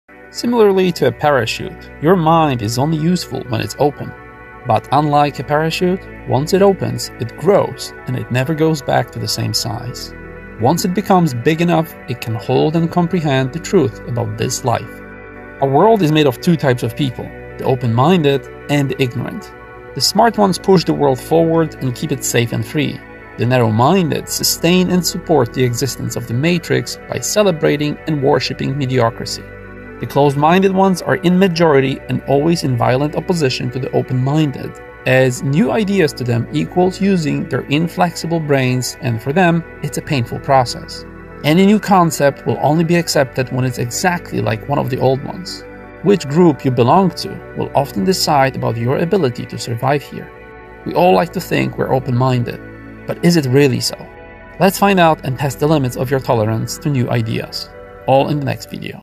Your mind is like a parachute
Similarly to a parachute, your mind is only useful when it's open. But unlike a parachute once it opens it grows and it never goes back to the same size. Once it becomes big enough it can hold and comprehend the truth about this life. Our world is made of two types of people: the open-minded and the ignorant. The smart ones push the world forward and keep it safe and free, the narrow-minded sustain and support the existence of the matrix by celebrating and worshipping mediocracy. The closed-minded ones are in majority and always in violent opposition to the open-minded as new ideas to them equals using their inflexible brains and for them, it's a painful process. Any new concept will only be accepted when it's exactly like one of the old ones. Which group you belong to will often decide about your ability to survive here. We all like to think we are open-minded but is it really so? Let's find out and test the limits of your tolerance to new ideas. All in the next video.